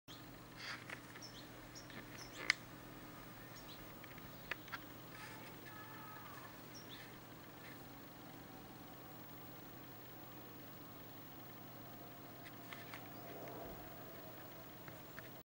Goldfinch feeding juveniles
Adult Goldfinch dropping seeds to juveniles.